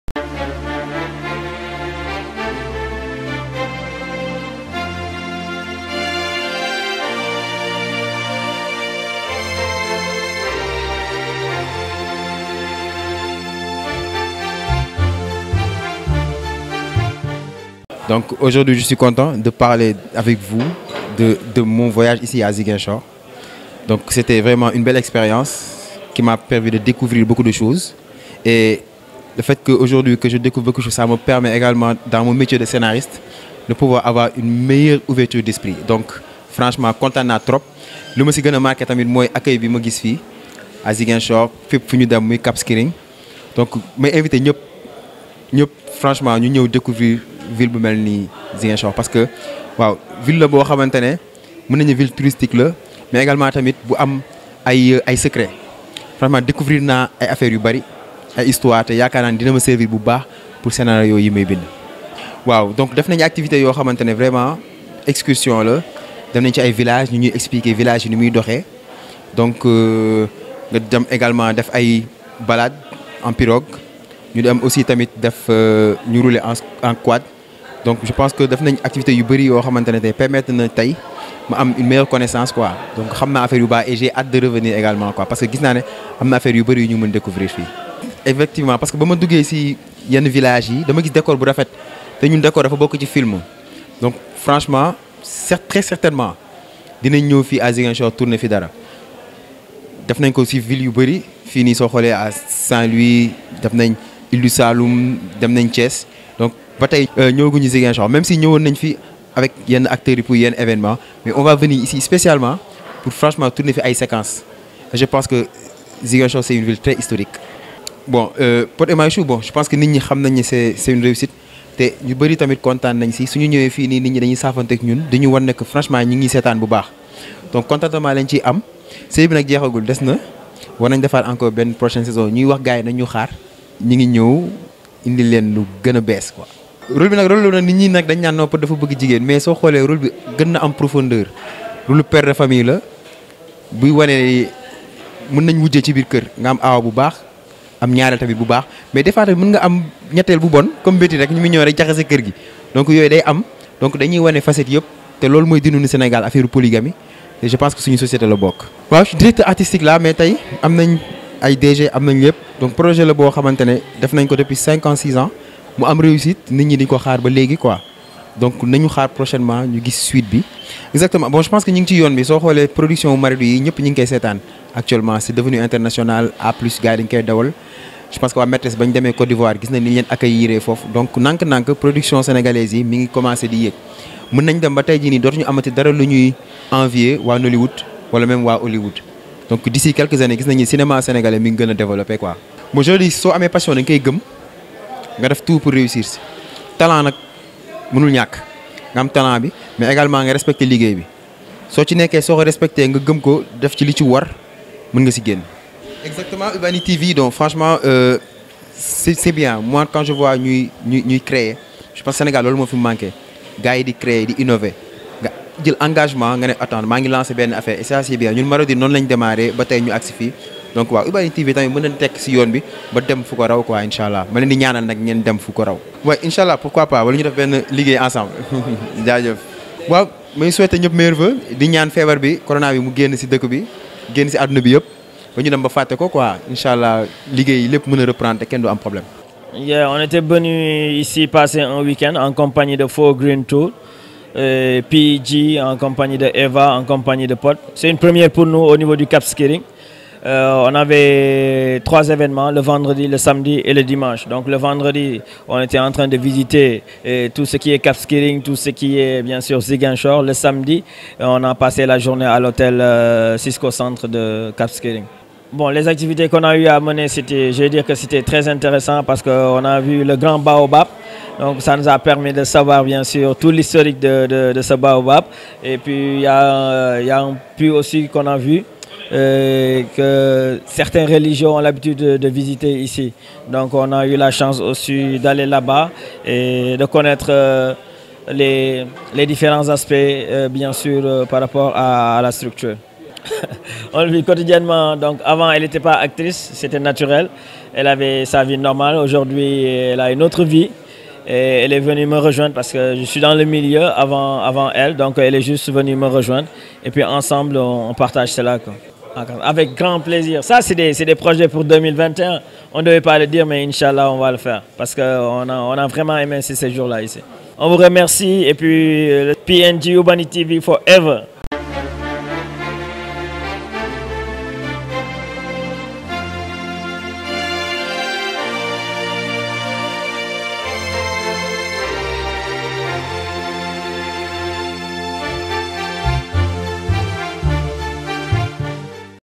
ZIGUINCHOR / TOURNAGE DE LA SERIE SENEGALAISE "POD ET MARICHOU" TRES PROCHAINEMENT DANS LA REGION
Ubani tv - A vous l'info